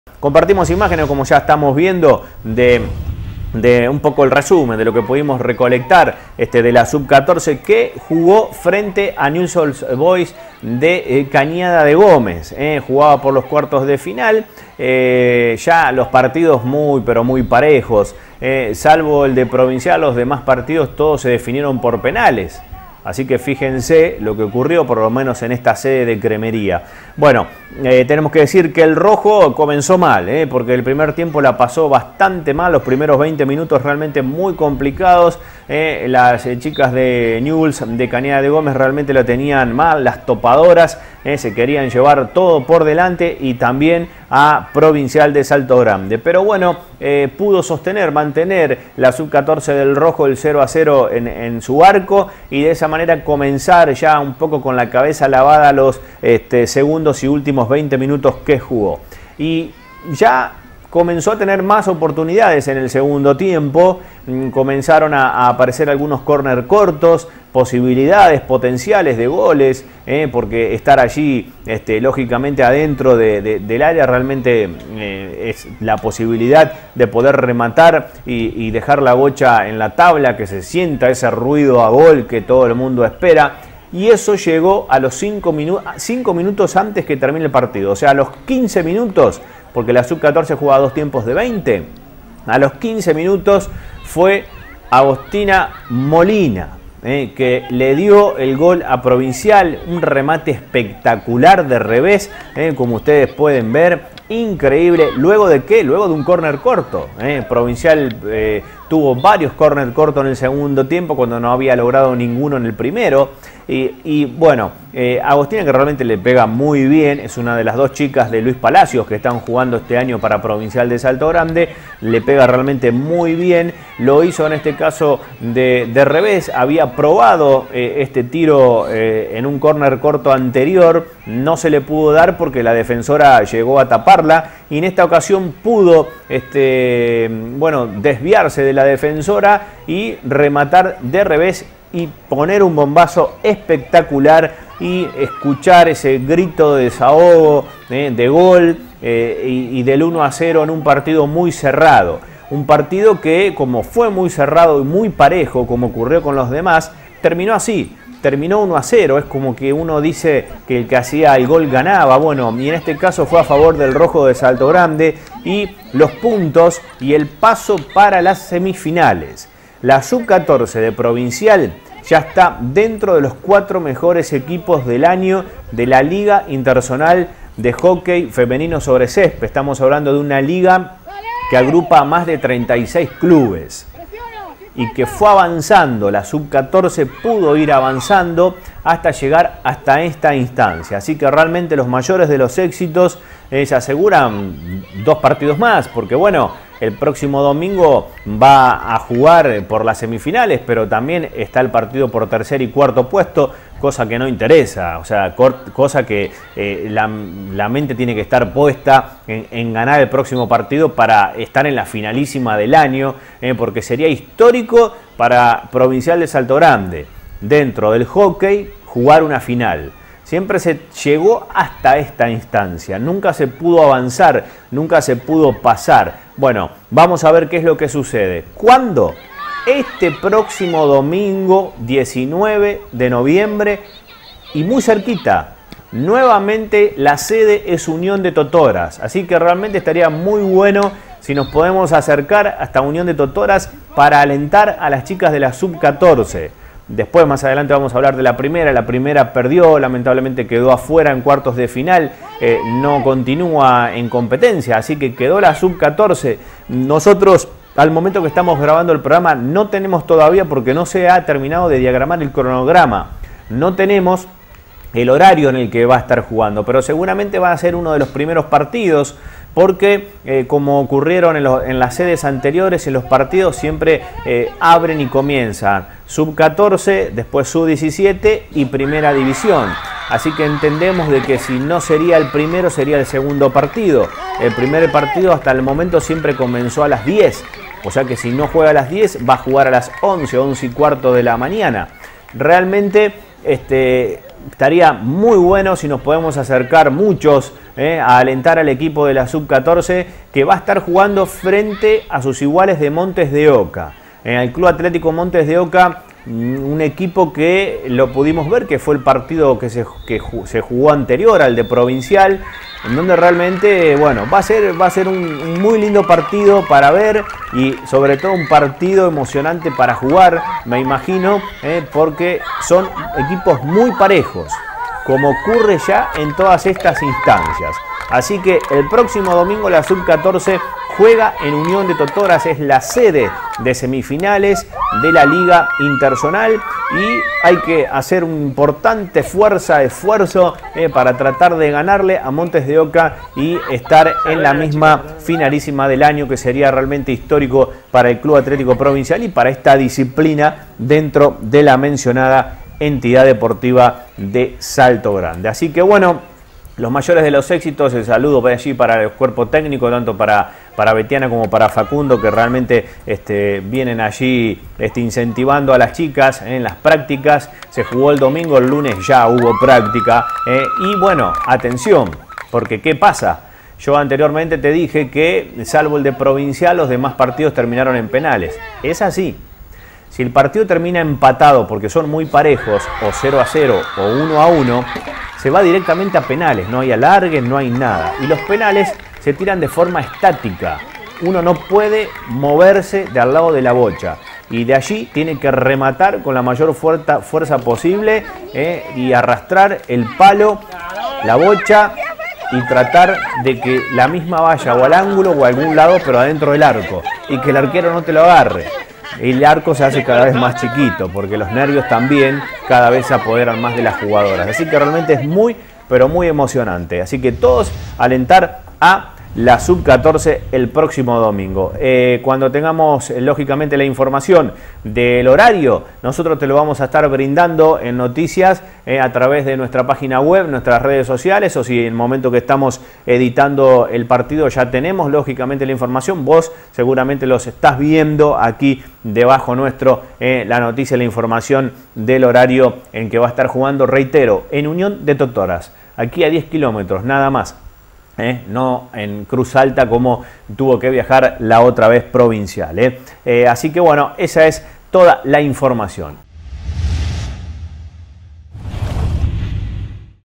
Info del Hockey y como se juegan las semis en UFC 17 11 2017 x264
Informe sobre la presentación y el triunfo de la categoría Sub 14 del Hockey Femenino del CAP por 1 a 0 frente a Newell s de Cañada de Gómez, que le dio el paso a las semifinales anuales del Torneo de la Liga Interzonal, que se jugarán este Domingo 19 de Noviembre en la sede de UFC de Totoras desde las 10 horas.
La Sub 14 jugará frente la semifinal frente a sus iguales de Montes de Oca desde las 11 horas.